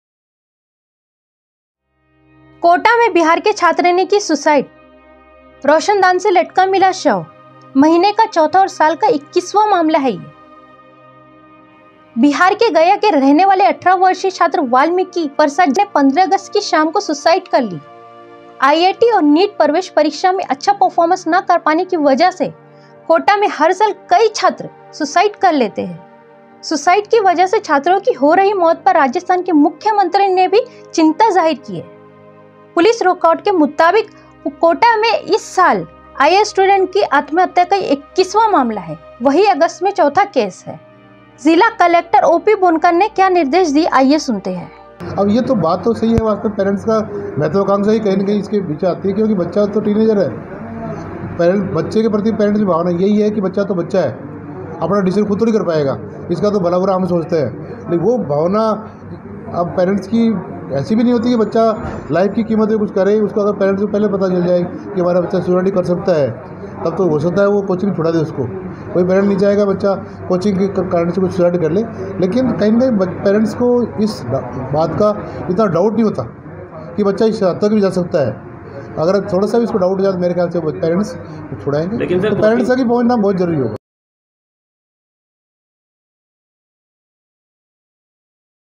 18 साल के छात्र ने किया सुसाइड, इस साल की यह 22वीं घटना I Aapka Prahar Times
18 साल के छात्र ने किया सुसाइड, इस साल की यह 22वीं घटना I Aapka Prahar Times